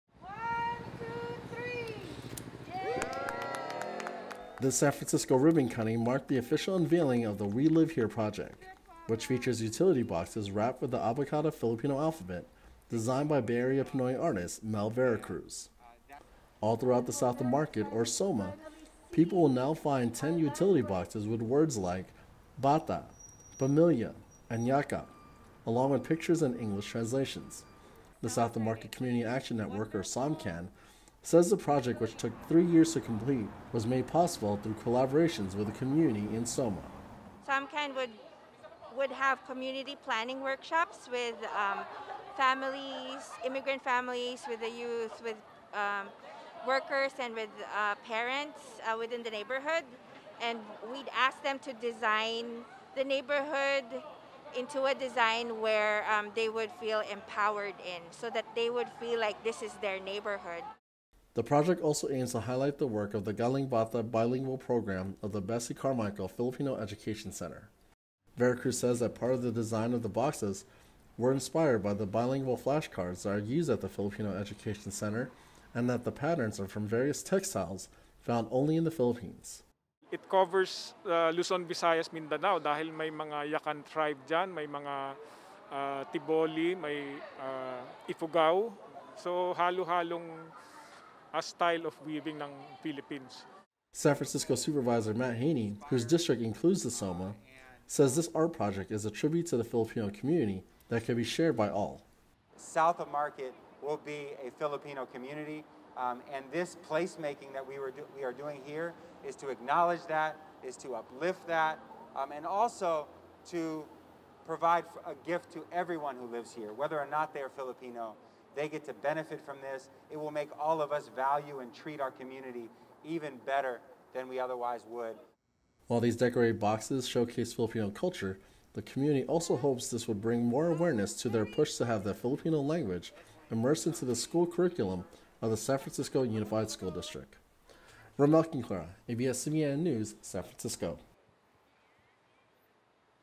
We Live Here project displays Filipino culture in San Francisco streets
Just in time for Filipino American History Month, San Francisco officially launches the art project that showcases Filipino language and culture through decorated utility boxes. Rommel Conclara takes us there with this story.